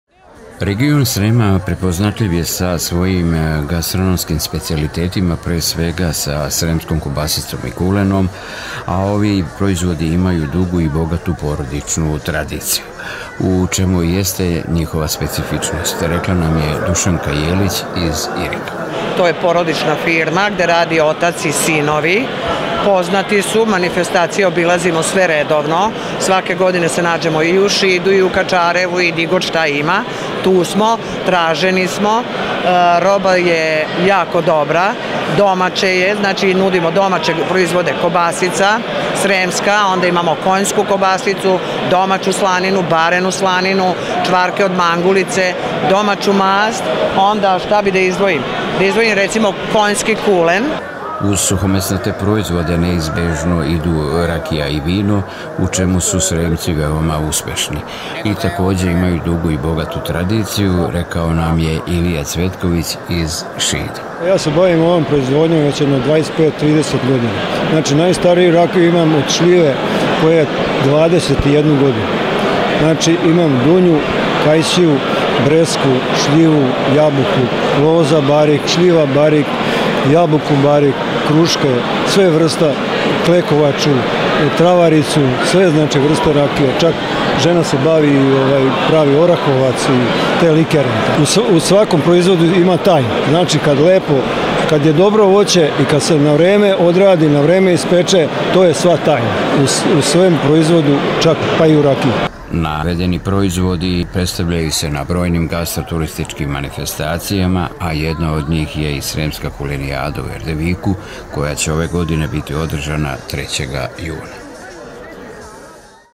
Tradicija i kvalitet u Sremu
Region Srema ima svoje administrativne granice koje su definisane sa opštinama odnosno lokalnim samoupravama, ali tu je i jedna tradicionalna granica. Reč je o Sremu koji je svinjski, vinski, rimski i ruzmarinski i ima svoja obeležja i karakteristike. Uz to treba istaći prepoznatljive sremske suhomesnate proizvode kvalitetna vina i rakije, a sve to dolazi iz porodičnih gazdinstava.